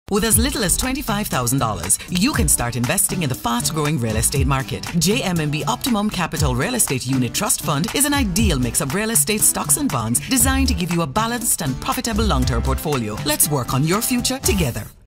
JMMB Optimum Capital (Real Estate) Fund
With as little as J$25,000 you can start investing in real estate with the JMMB Optimum Capital (Real Estate) Unit Trust Fund. Please Contact a JMMB representative at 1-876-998-JMMB (5662) for more information.

*Conditions Apply.